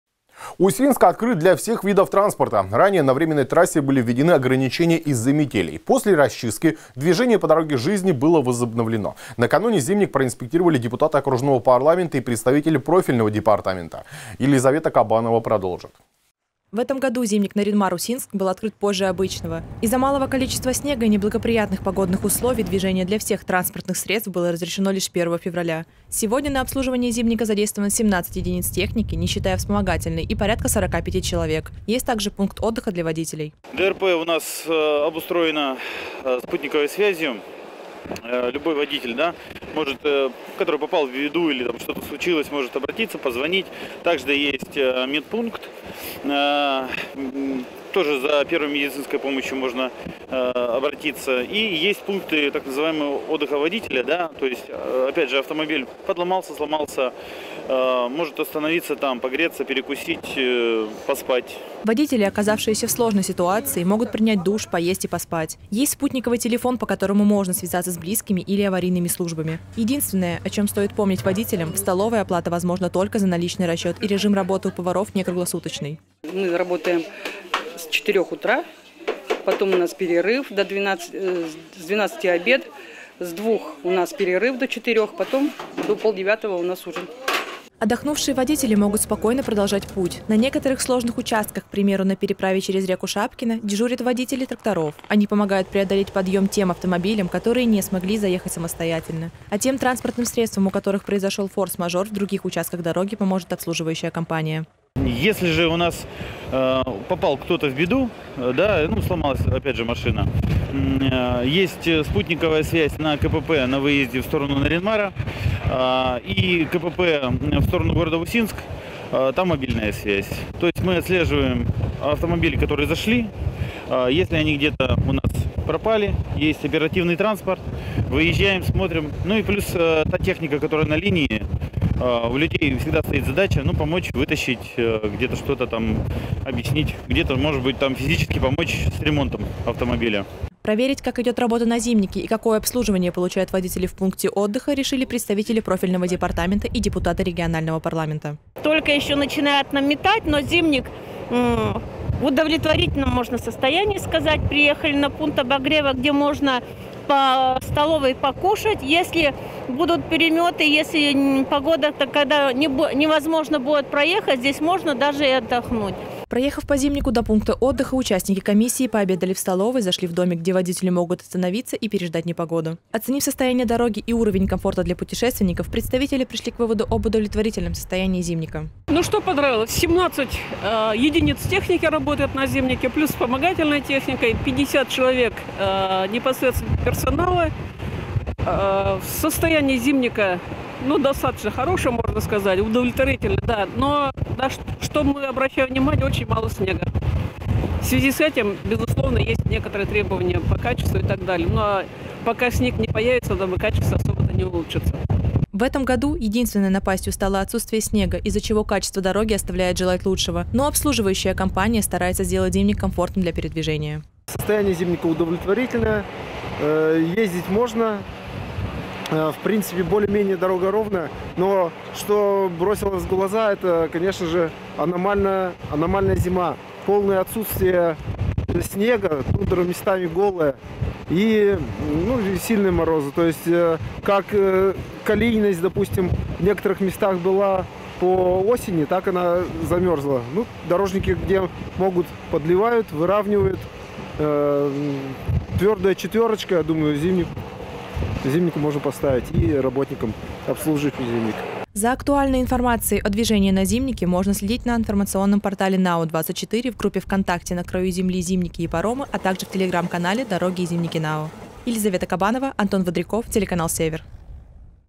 ЗИМНИК ОТКРЫТ
Зимник Нарьян-Мар - Усинск открыт для всех видов транспорта. Ранее на временной трассе были введены ограничения из-за метелей. После расчистки движение по дороге жизни было возобновлено. Накануне зимник проинспектировали депутаты окружного парламента и представители профильного департамента.